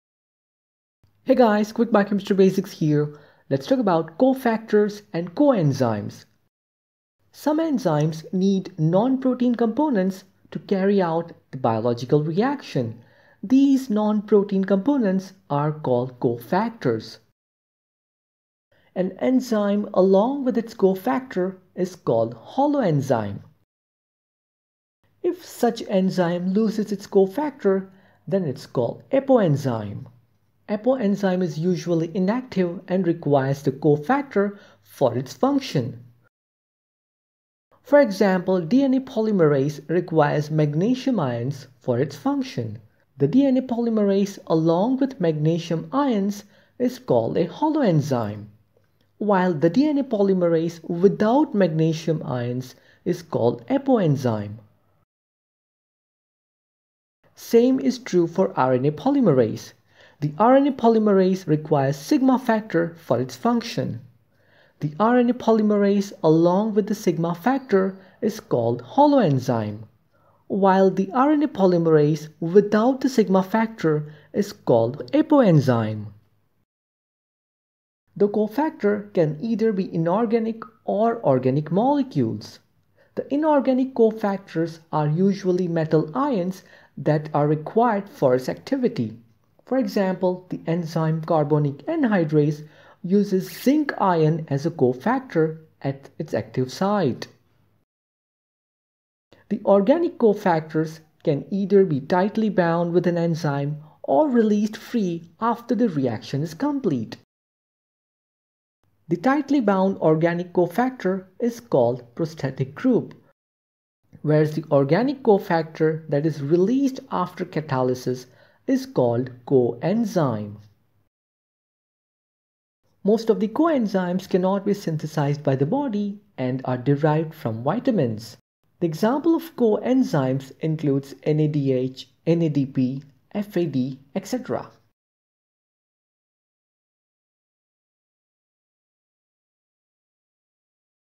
Cofactors | Coenzymes | Holoenzyme | Apoenzyme
Some enzymes needs non protein components to carry out biological reactions these non protein components are called cofactors. An enzyme along with its cofactor is called holoenzyme. If such enzyme looses its cofactor then its called apoenzyme. The cofactors can either be inorganic or organic molecules. The inoganic confactors are usually metal ions that are requried for its activity. The organic cofactors  can either be tightly bound with the enzyme or released free after the reaction is complete. The tightly bound organic cofactor is called prosthetic group where as the organic cofactor that is releasd after catalysis is called coenzyme.